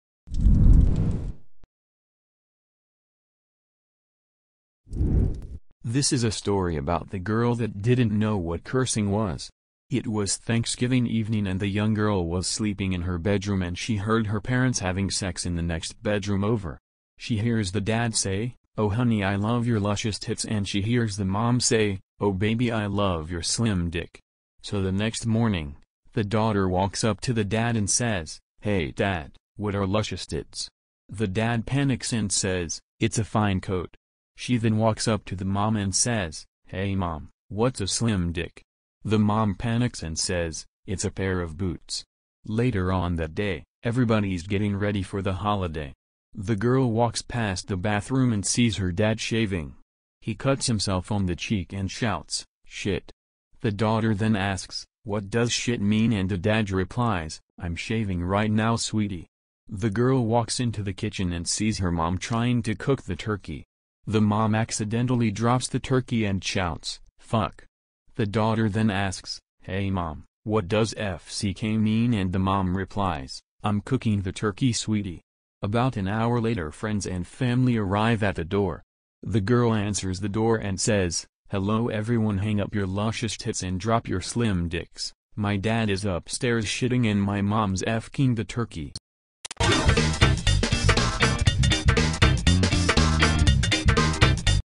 funny joke_ This is a story about the girl that didn't know what cursing was
This is a story about the girl that didn't know what cursing was. It was Thanksgiving evening and the young girl was sleeping in her bedroom and she heard her parents having sex in the next bedroom over. She hears the dad say, "oh honey I love your luscious tits" and she hears the mom say, "oh baby I love your slim dick". So the next morning, the daughter walks up to the dad and says, "Hey dad, what are luscious tits?" the dad panics and says, "It's a fine coat". She then walks up to the mom and says, "Hey mom, what's a slim dick?". The mom panics and says, "It's a pair of boots". Later on that day, everybody's getting ready for the Holiday. The girl walks past the bathroom and sees her dad shaving. He cuts himself on the cheek and shouts, "Shit!". The daughter then asks,"What does shit mean" and the dad replies, "I'm shaving right now sweety". The girl walks into the kitchen and sees her mom trying to cook the turkey. The mom accidentally drops the turkey and shouts, "Fuck". The daughter then asks, "hey mom, what does f*ck mean" and the mom replies, "I'm cooking the turkey sweety". About an hour later friends and family arrive at the door. The girl answers the door and says, "Hello everyone hang up your luscious tits and drop your slim dicks, my dad is upstairs shitting and my mom's f*cking the turkey".